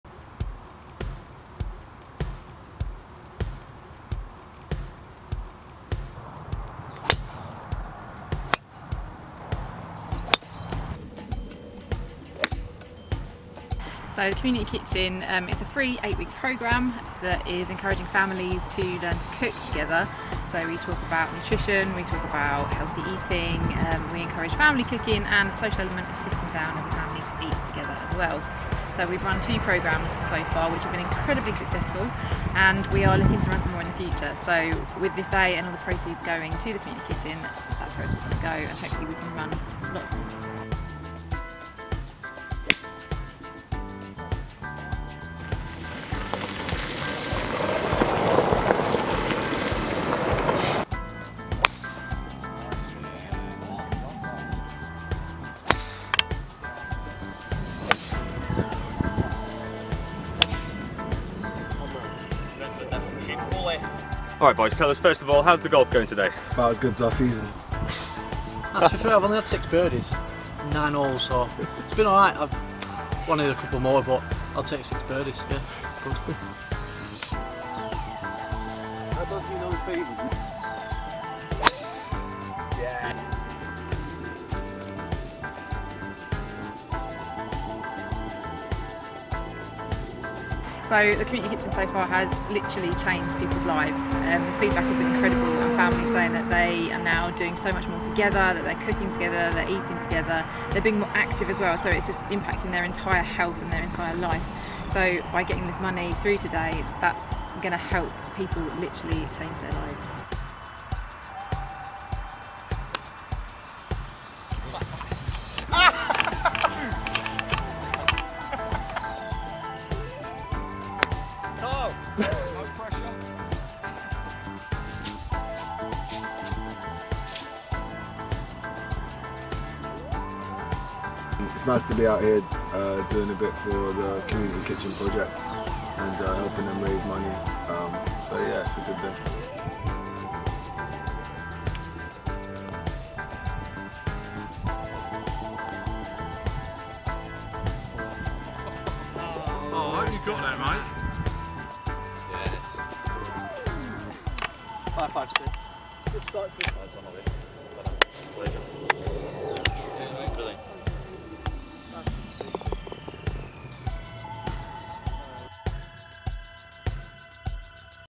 2019 Stevenage FC Foundation Golf Day
The 2019 Stevenage FC Foundation Golf Day helped raise money for the Community Kitchen Project.